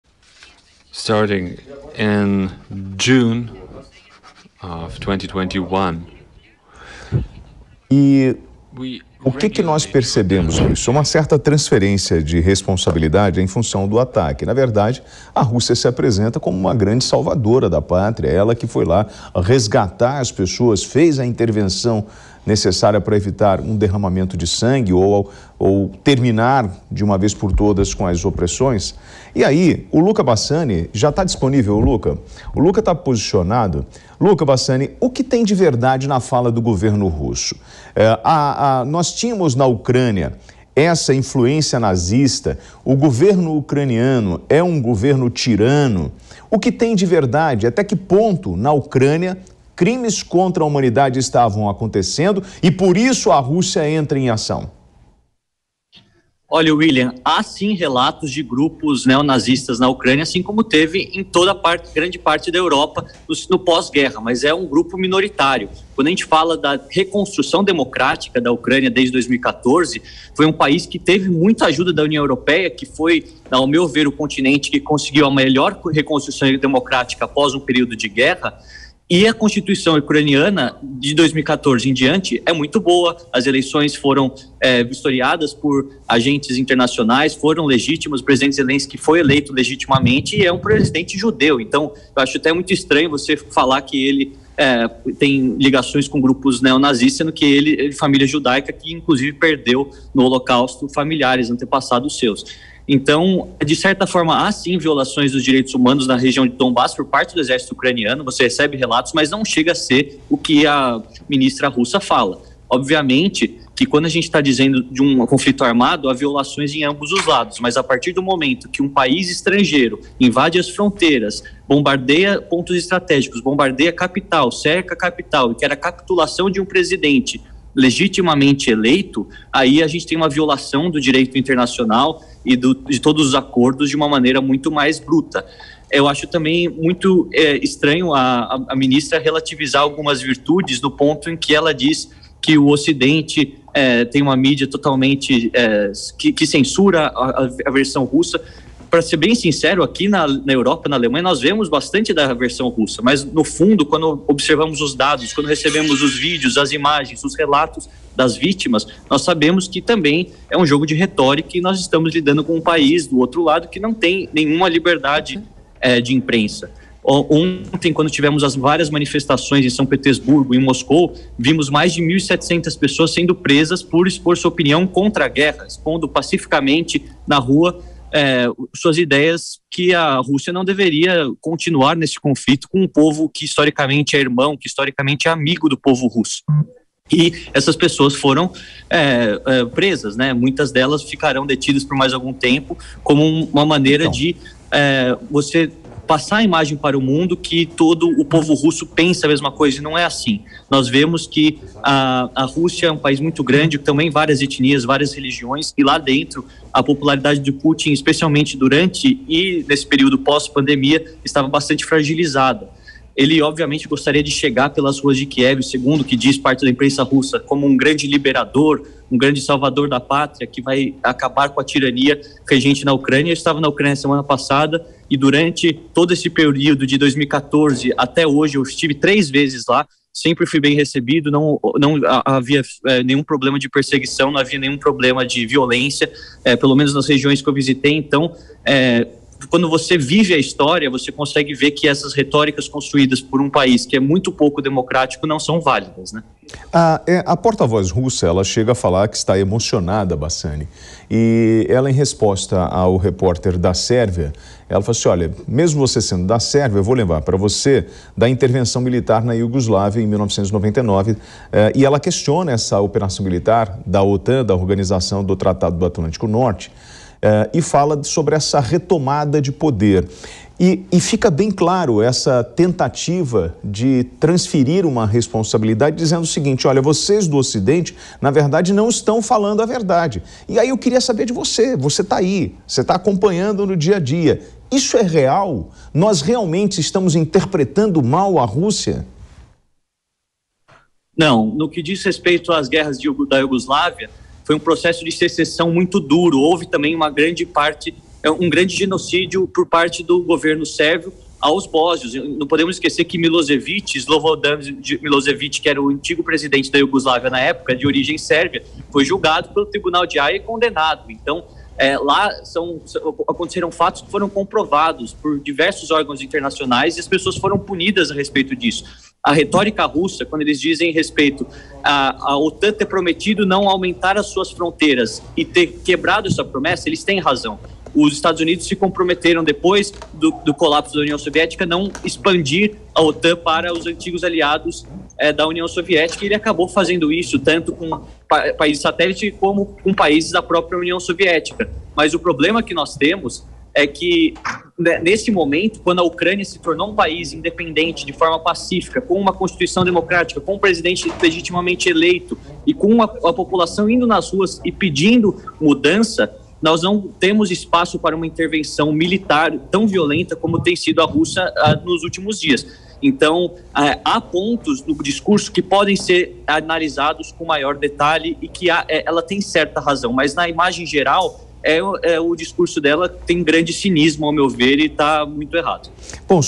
Porta-voz da Rússia critica ‘histeria’ do Ocidente e diz que civis não foram atacados
No segundo dia de ataques russos à cidades da Ucrânia, a porta-voz do Ministério das Relações Exteriores da Rússia, Maria Zakharova, criticou a “histeria” criada pelo Ocidente e a cobertura dada pela imprensa ao episódio.